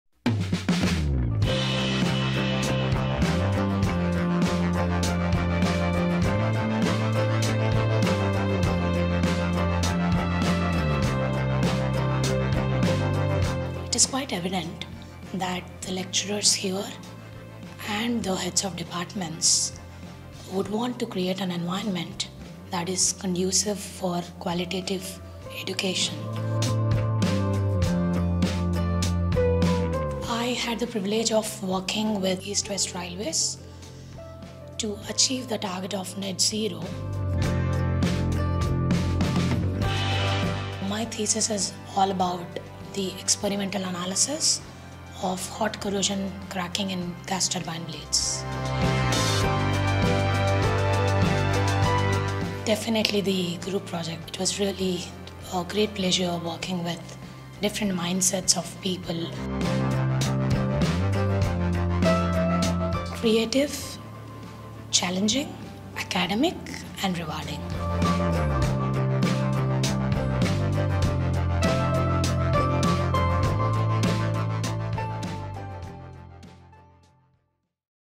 Why study Advanced Mechanical Engineering MSc? - Supriya Marripalli
Current student Supriya Marripalli talks about the Advanced Mechanical Engineering MSc